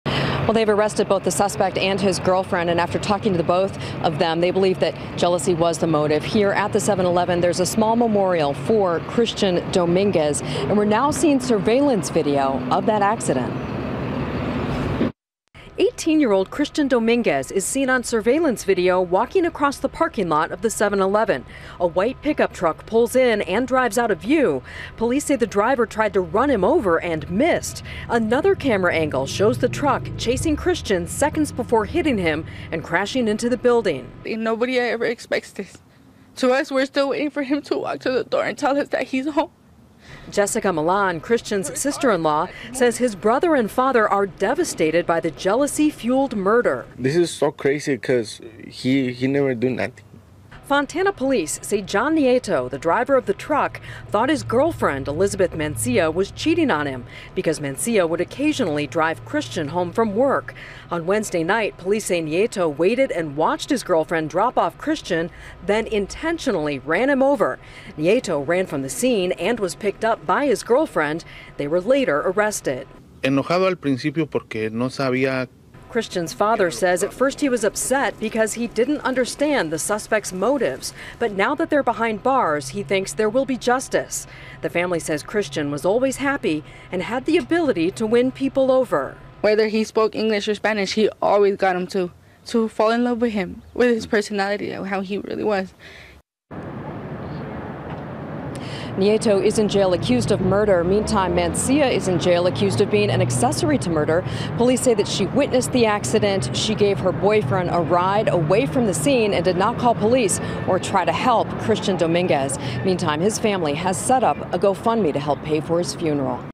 Surveillance video shows moments before Fontana jealousy-fueled murder - ABC7
Surveillance video from outside a Fontana 7-Eleven shows the moments leading up to what police say was a jealousy-fueled murder.